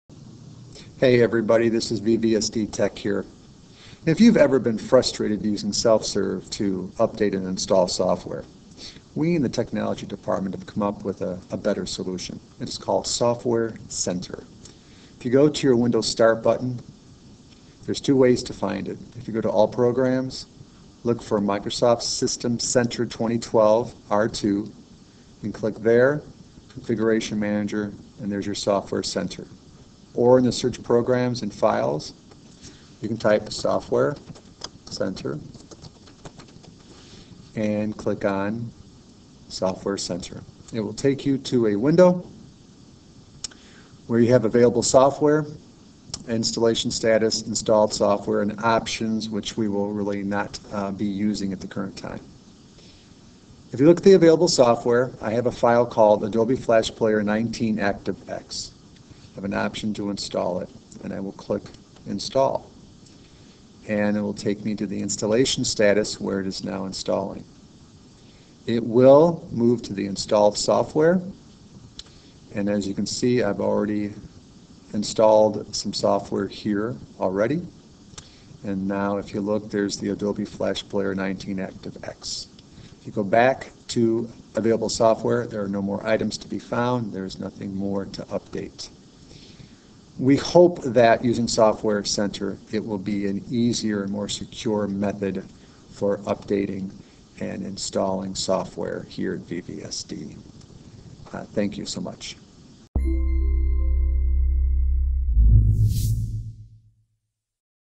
Software Center
While using the software center on Windows isn't primarily a Google item, you can use the Software Center to update applications on your computer including the Google Chrome browser!